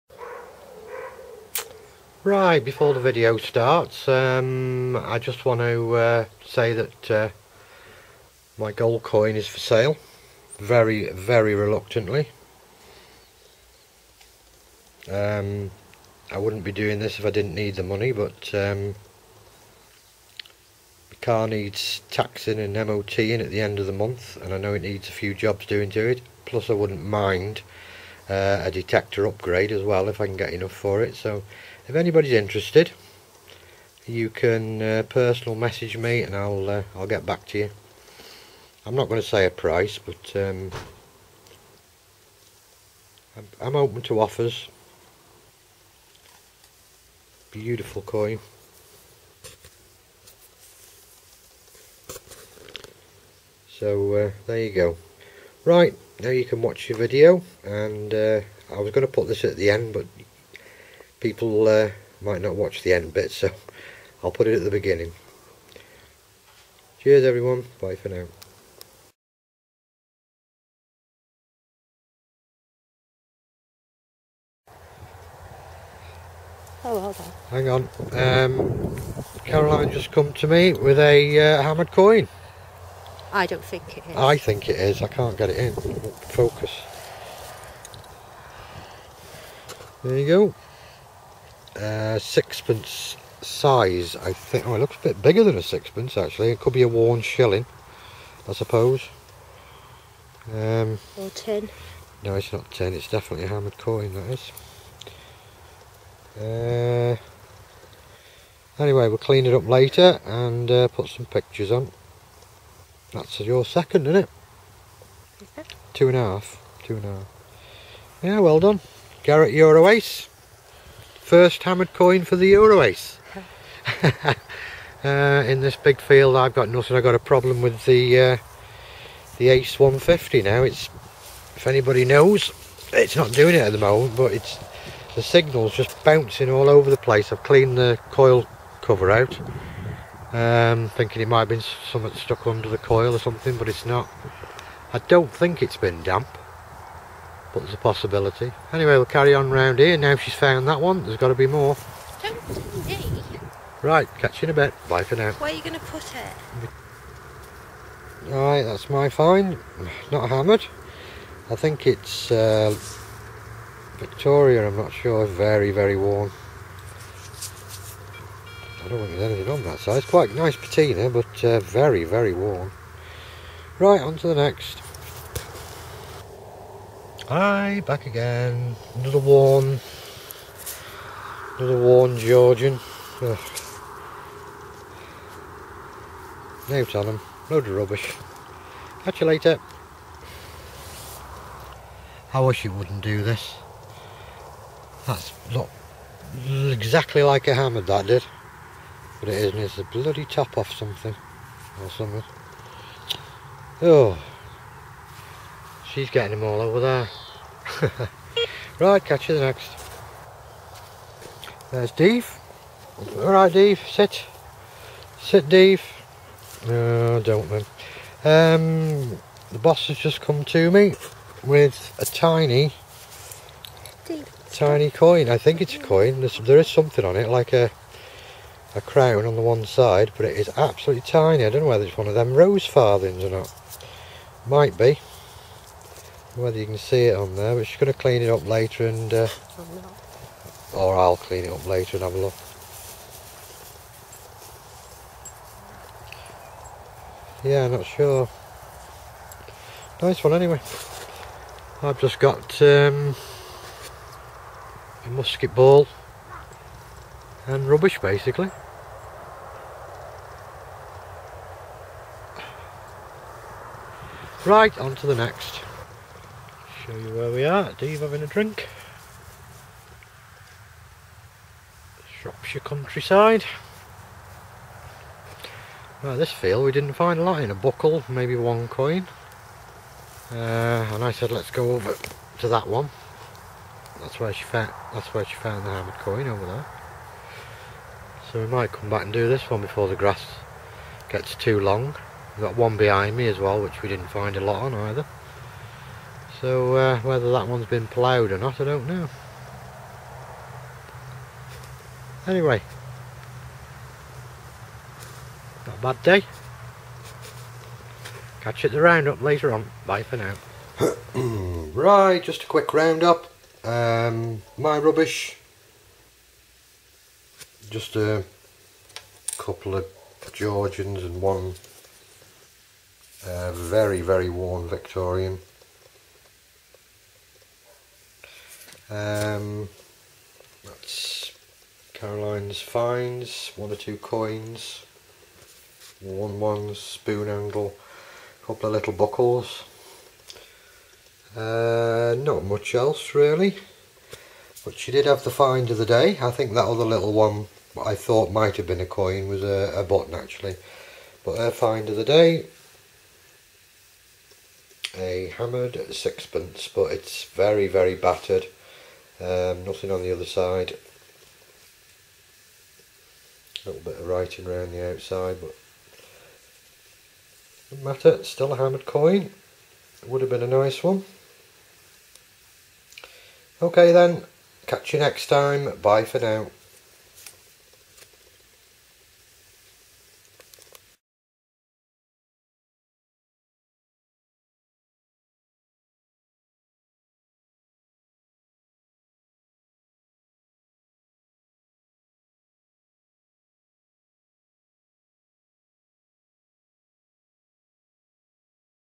CAROLINE'S EUROACE FINDS HAMMERED SILVER....PLUS MY GOLD COIN IS FOR SALE
today we went to some fields that we have not done much,but a silver hammered sixpence turns up for my wife...her first one with the euroace..